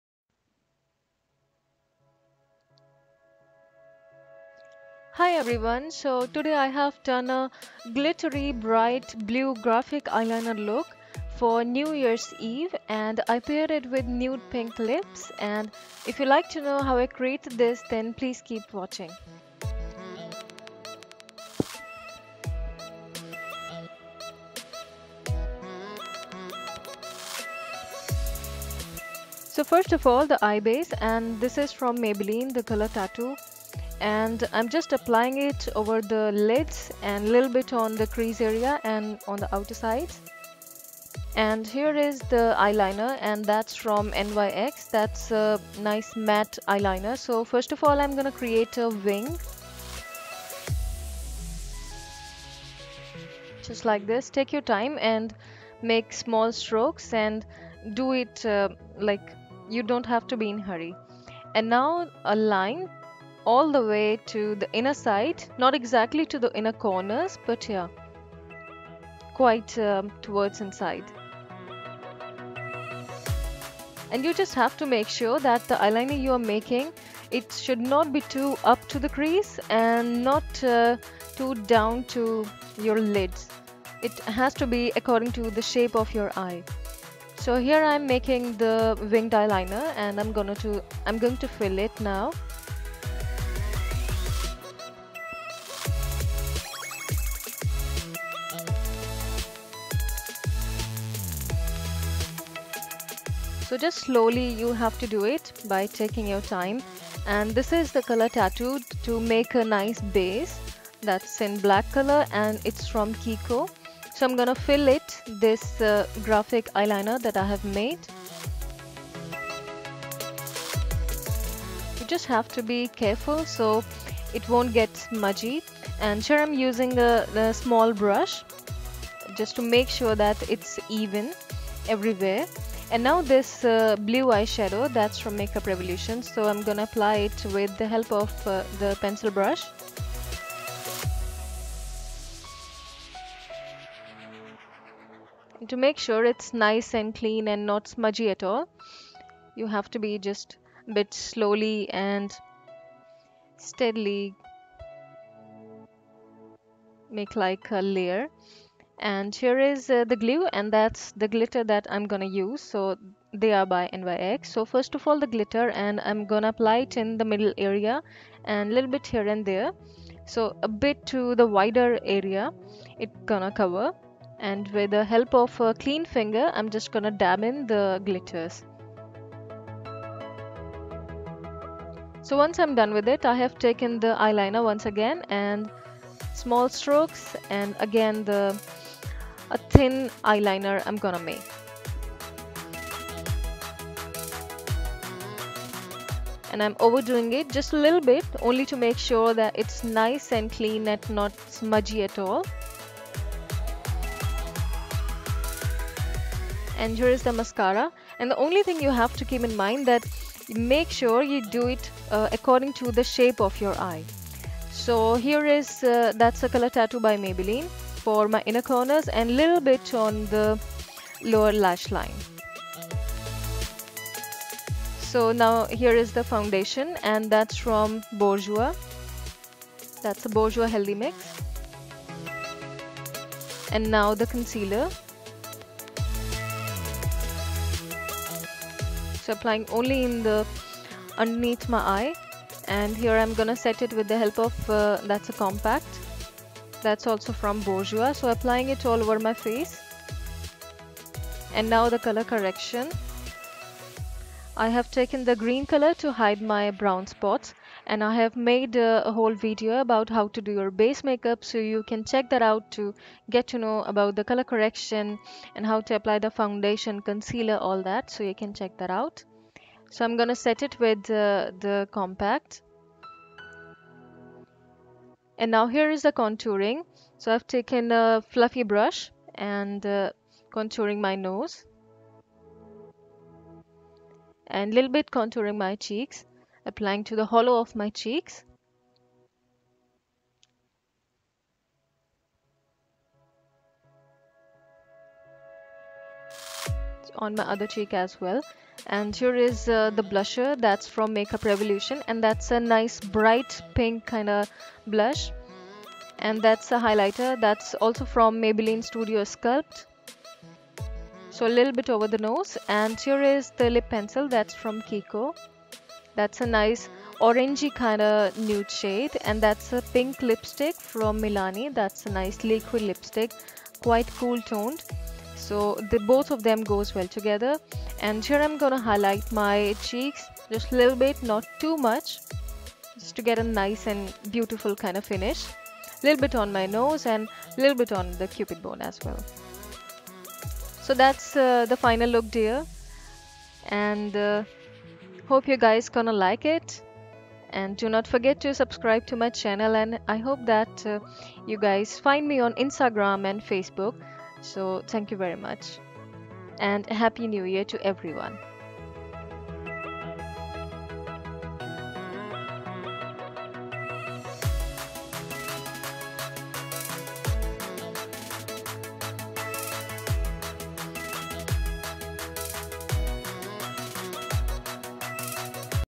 GRAPHIC EYELINER With Glitter Makeup Tutorial | New Years Eve Look
Hi everyone, so today I have done a new year's evening makeup tutorial. I have applied blue glitters and made a graphic eyeliner, paired it with nude pink matte lips. Hope you guys like it :)

Below is the list of products I have used to create the look :)

1. Foundation Bourjois Healthy Mix.
3. Eyeshadows by Make up Revolution Palette, NYX Loose glitter in Blue.
4. Eye Base maybelline color tattoo.
5. Maybelline concealer no.3 & MAC mineralized NC 42
6. Maybelline volum express colossal mascara
7. Blush by Makeup Revolution.
8. Contouring palette Maybelline Master Sculpt
9. Compact by bourjois healthy balance
10. Lipstick Rimmel Lip liner in Oil Spice and Mlani Amore Matte Liquid Lipstick in Fling.
11. Eyebrow Pencil by Rimmel
12. Eye lashes by Eyelure.
14. Color corrector by Makeup Revolution.
15.. Eyelashes by eyelure.